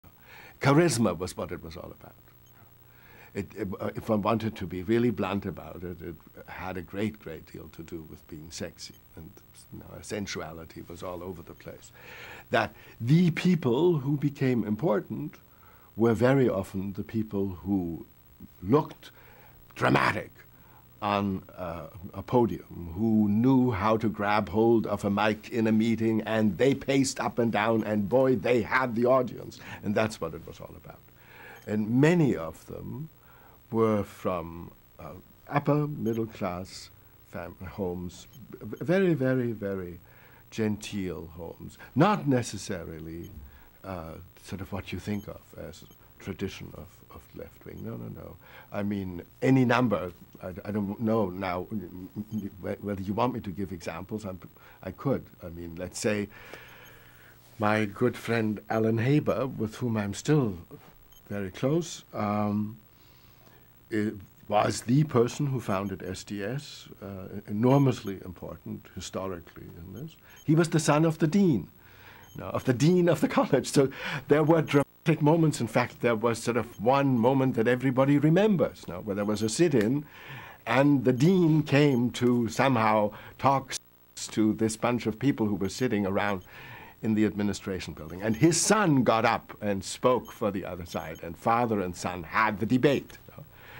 The 1960s Leaders Had Charisma & Great Looks
To support my efforts to create more clips please donate to me The speaker is Frithjof Bergmann the famed professor of philosophy at the University of Michigan. He was an articulate witness to the changes that took place in America and especially on universities in the 1960s. He went on to study blue-collar workers at the automobile plants in Michigan and developed a concept for work he called “new work”. He also wrote the popular books “On Being Free” and “The Experience Of Values”. I interviewed him in 1989 for my TV series looking at the 1960s. I remember at the time of the interview that virtually every answer he gave to my questions was fascinatingly unique.